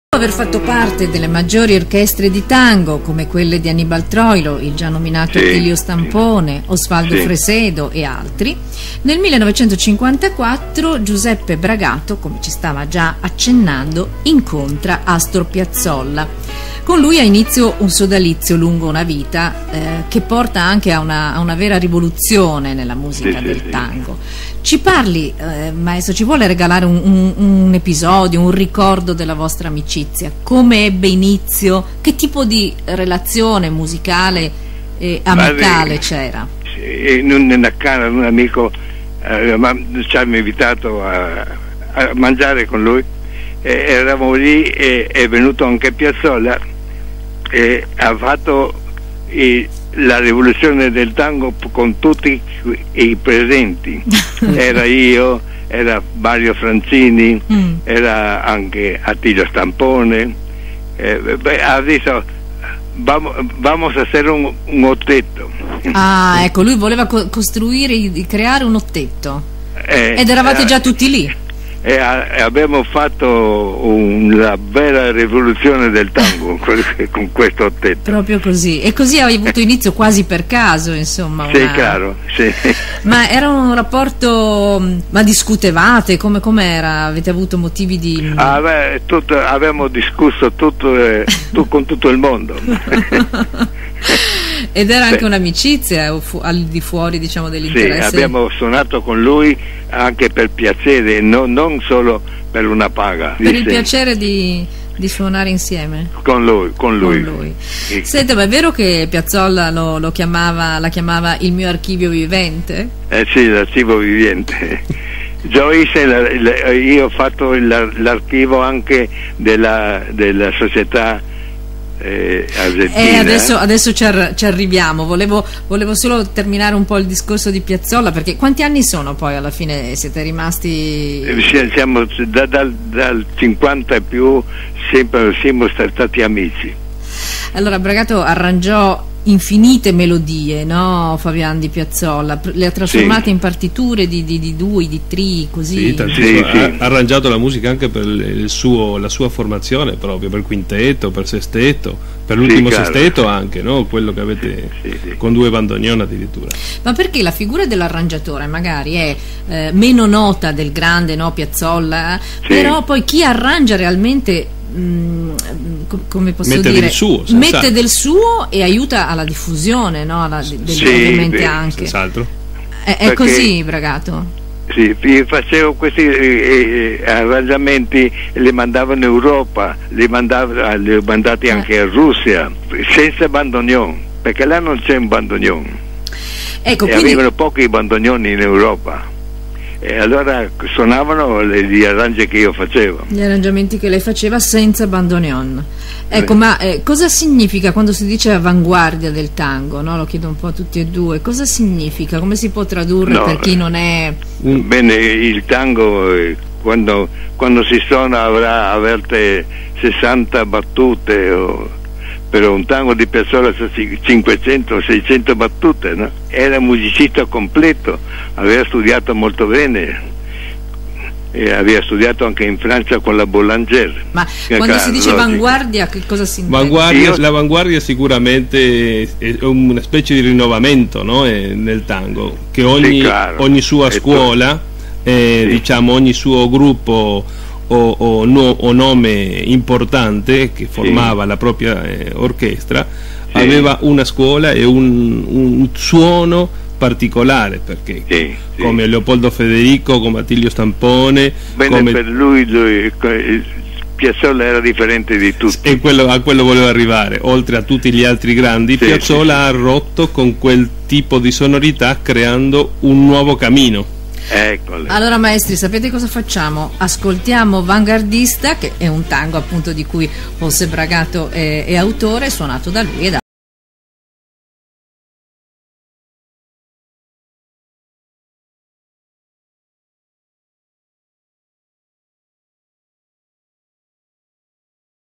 JOSE BRAGATO REPORTAJE 2008 TERCERA PARTE
A José Bragato le realizaron un extenso reportaje desde Trieste en el 2008, a raíz del premio Due Mondi, que Walter Mattiuzzi, residente en Udine, ciudad natal del maestro violoncellista, le otorgó. He aquí la tercera parte.